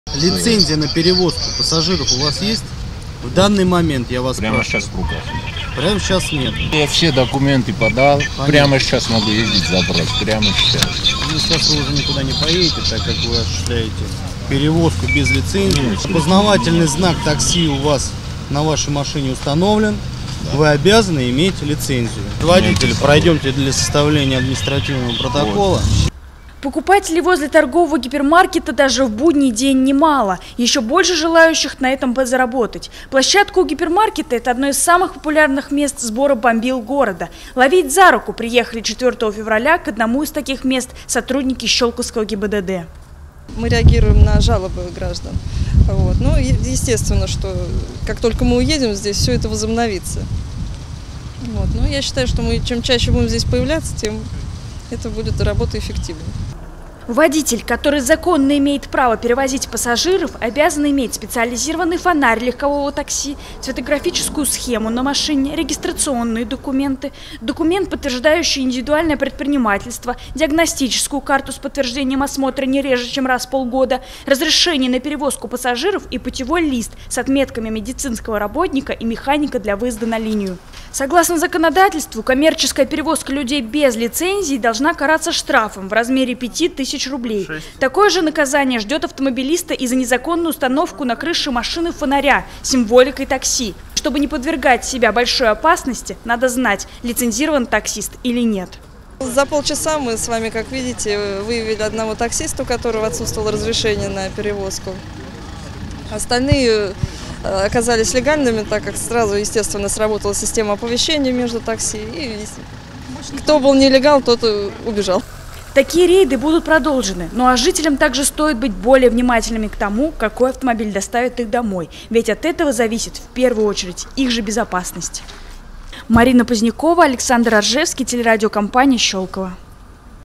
Сотрудники ГИБДД провели рейд по выявлению нелегально работающих таксистов
Нелегалам-стоп! Сотрудники Щёлковского отдела  ГИБДД провели рейд по выявлению нелегально работающих таксистов. Местом проведения стала площадка крупного гипермаркета